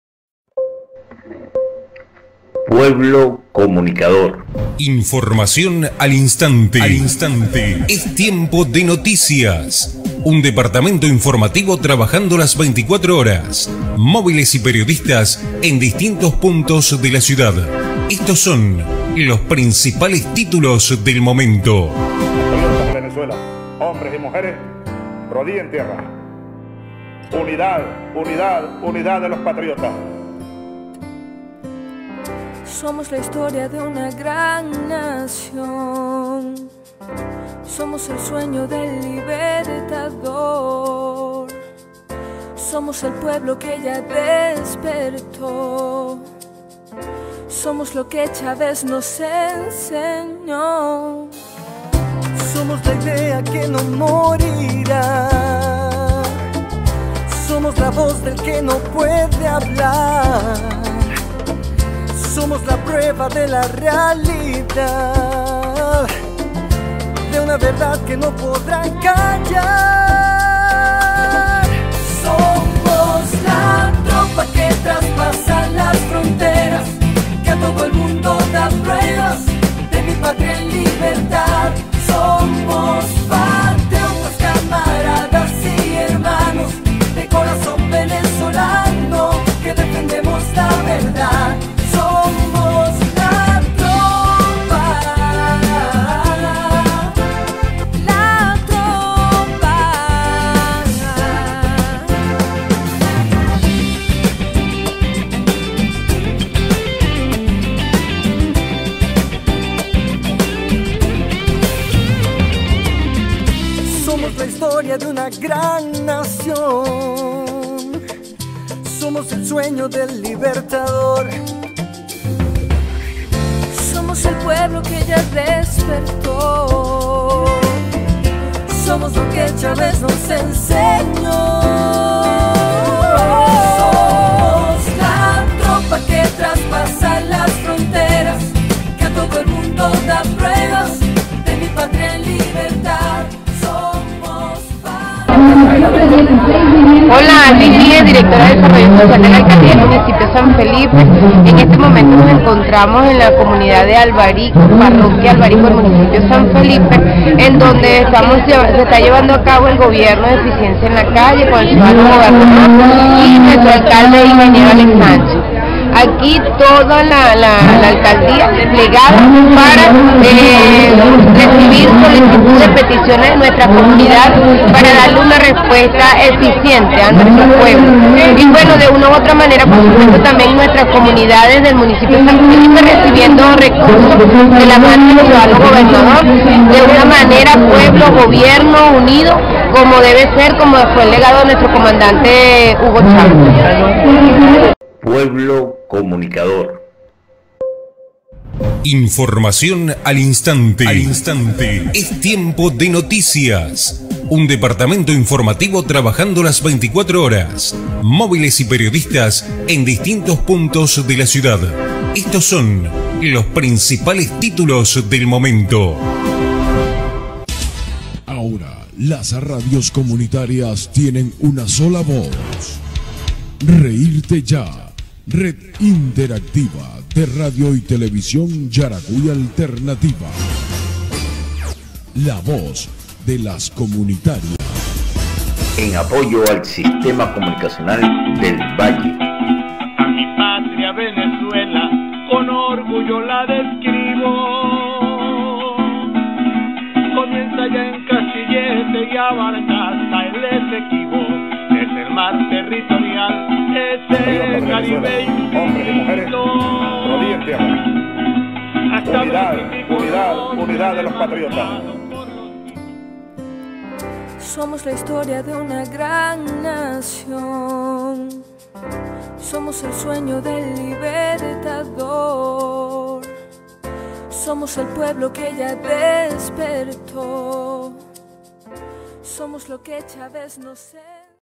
#‎EsTiempoDeNoticias‬ Gobierno de Calle, Comentario de: Alvis America Viez
‪#‎EsTiempoDeNoticias‬ Gobierno de Calle con Julio León, Comentario de: Alvis America Viez, Directora Desarrollo Social de la Alcaldia de San Felipe